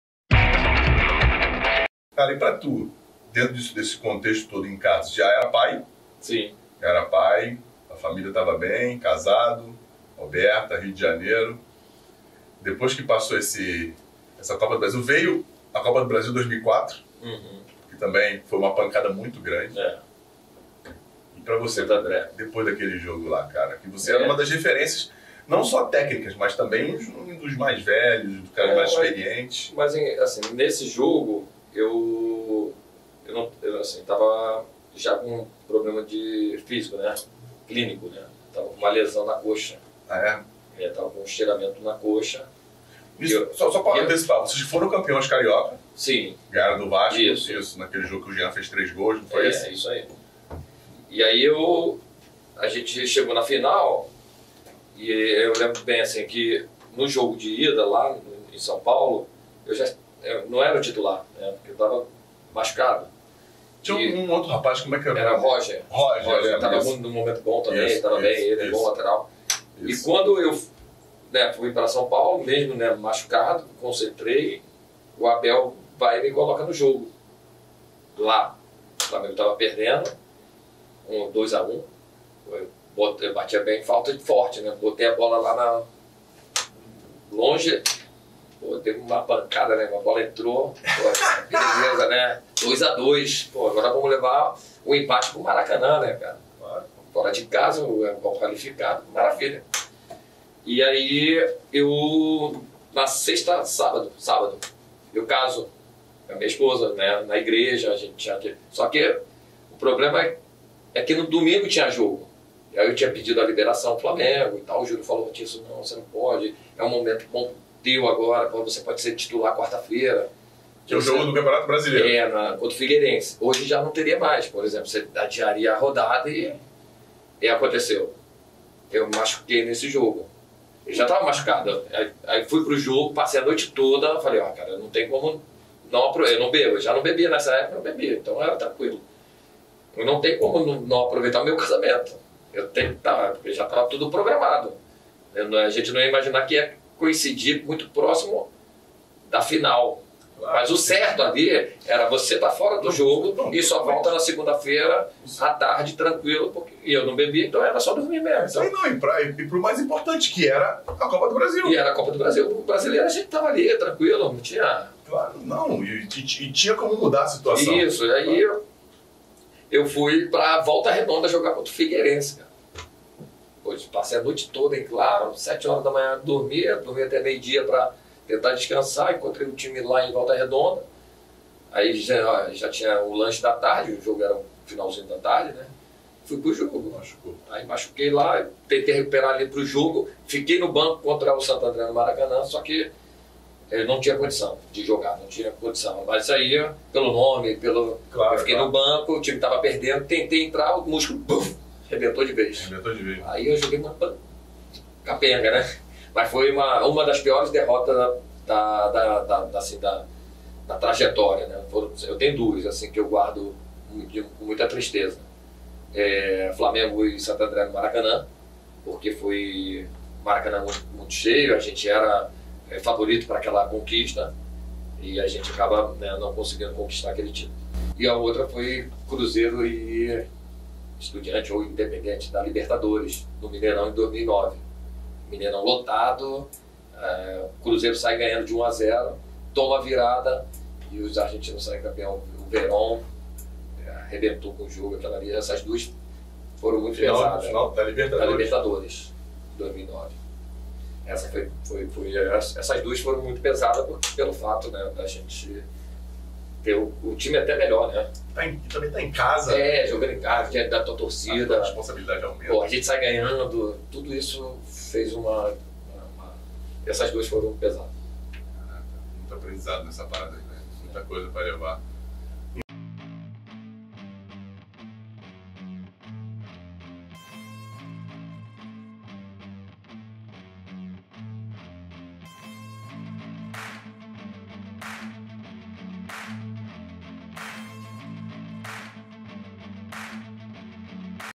DO FAVORITISMO A FRUSTRAÇÃO. AS DERROTAS MAIS MARCANTES DA CARREIRA DE ATHIRSON.
Esse é o canal oficial de cortes do Storicast.

Oba oba, galera.

Sejam bem vindos a nossa fogueira.

No episódio de hoje Fernando Santos e Anselmo Paiva recebem o ex atleta e atual comentarista Athirson, com passagem por vários times do Brasil, como Flamengo, Santos, Botafogo e Cruzeiro, no futebol europeu passou por Juventus e Bayer Leverkusen. Em uma resenha leve e descontraída ele relembra sua carreira como e jogador e treinador, conta detalhes sobre final da Copa do Brasil de 2004 e da final da Taça Libertadores de 2009, retomada na Portuguesa, lesões e aposentadoria.

IMPERDÍVEL!